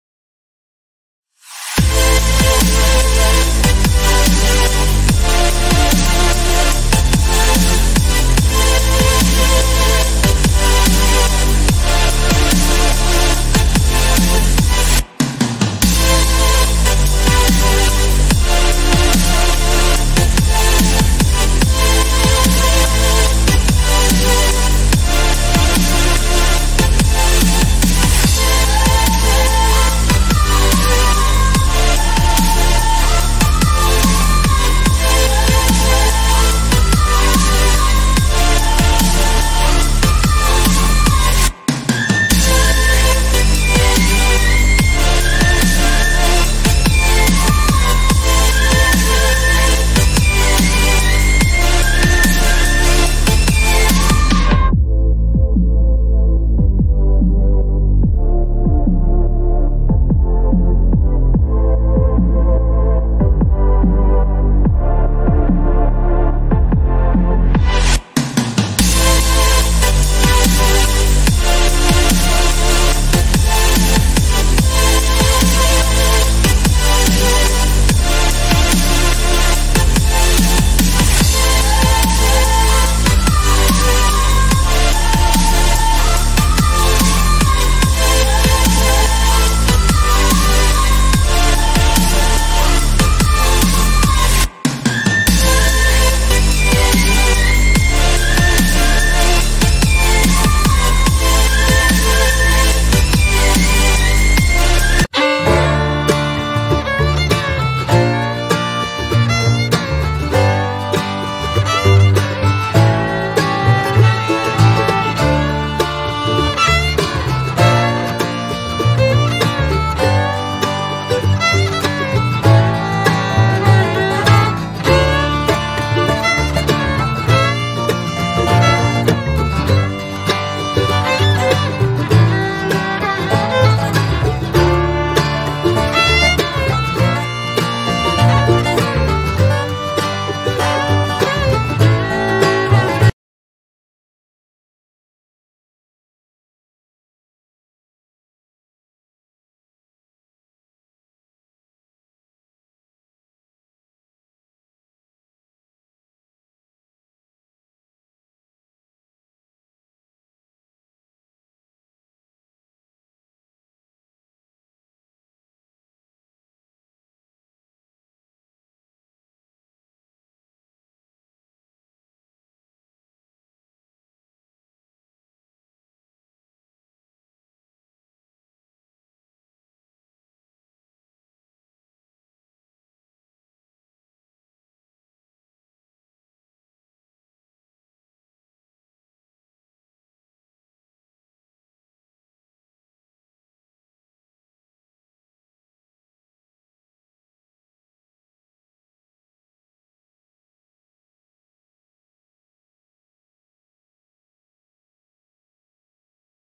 Diyar El Sidik Hotel Apartments
Located In Medina In The Al Madinah Region, Diyar El Sidik Hotel Apartments Provides Accommodations With Free Wifi And Free Private Parking.
Each Unit Has A Fully Equipped Kitchen With A Fridge, A Seating Area With A Sofa, A Flat-Screen TV And A Private Bathroom With Shower And Free Toiletries.
Al-Masjid An-Nabawi Is 12.9 Km From The Condo Hotel, While Miqat Mosque Is 3.1 Km Away. The Nearest Airport Is Prince Mohammad Bin Abdulaziz International.

Share And Like My Videos Please do comments and sharing your experience during the stay in hotel for new guests.